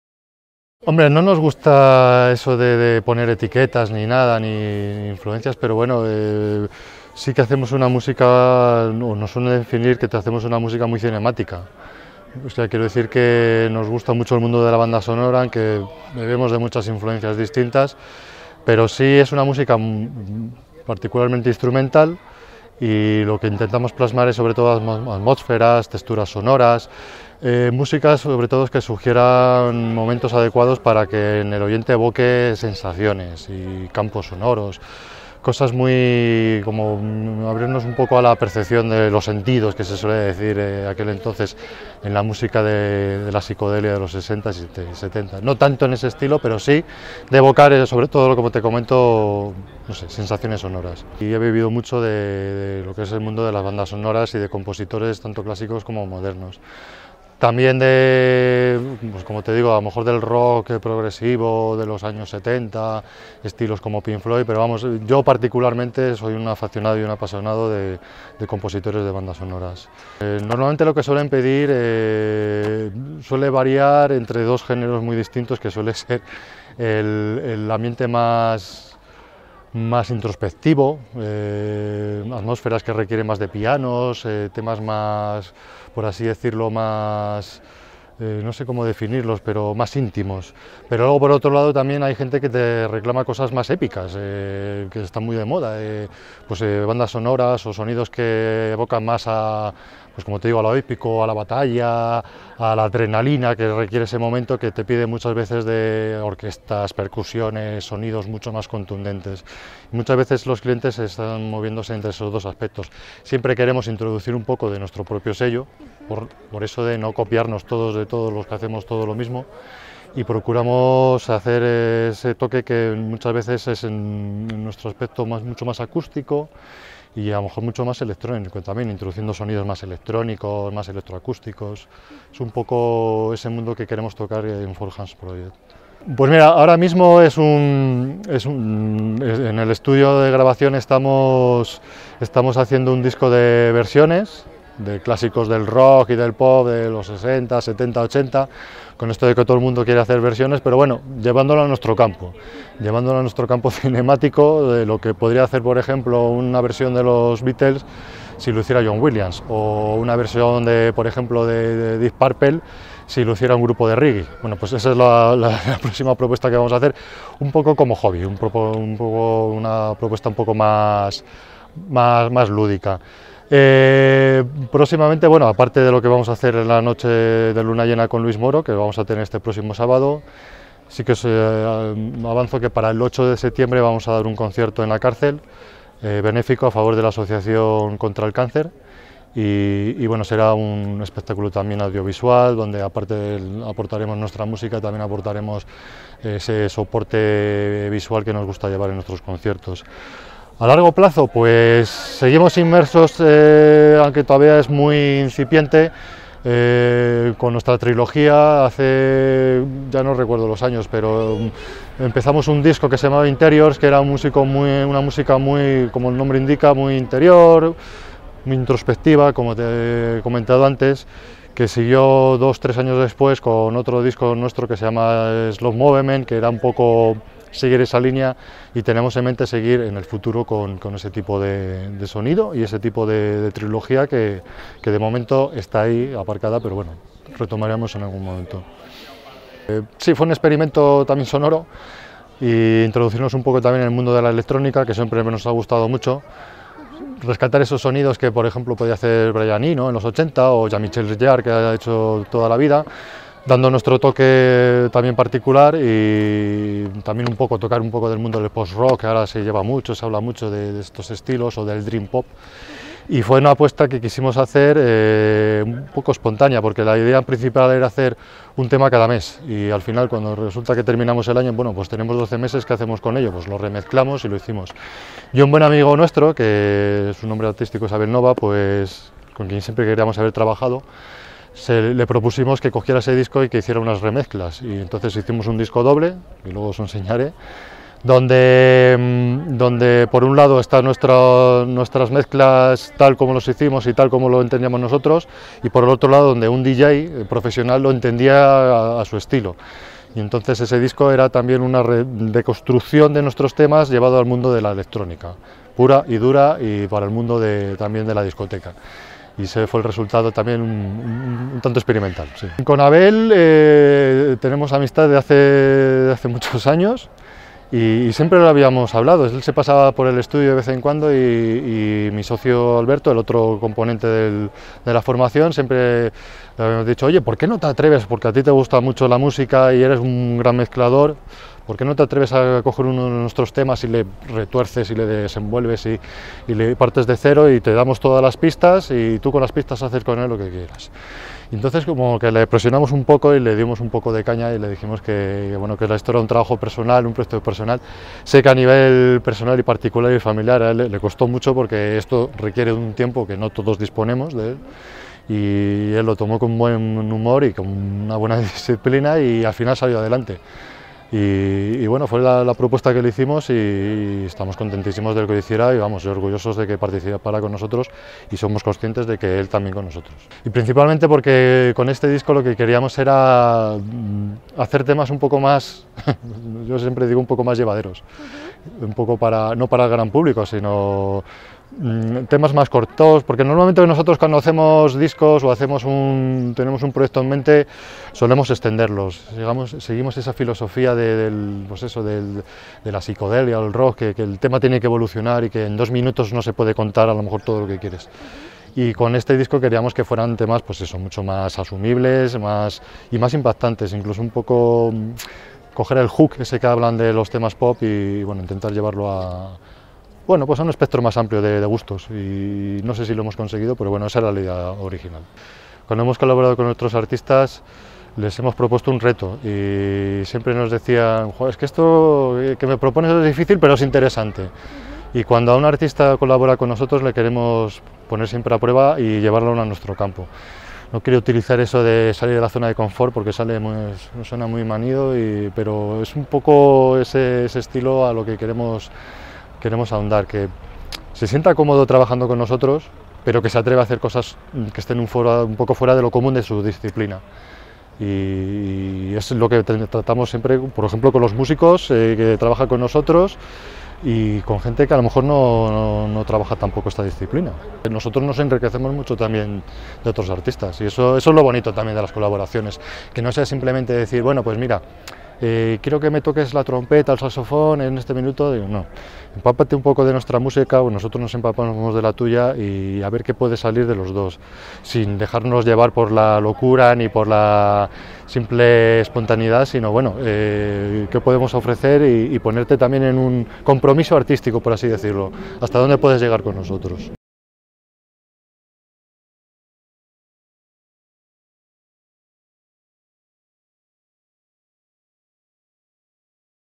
David Gómez (Four Hands Project): "Sensaciones sonoras que evocan a los sentidos"
David Gómez es uno de los integrantes, junto con Alberto Ayuso, del dúo segoviano de compositores Four Hands Project. Tras el éxito que ha tenido su espectáculo con Luis Moro en la Noche de Luna Llena, David, director creativo, nos habla sobre su música y los proyectos en los que están inmersos en la actualidad y a futuro.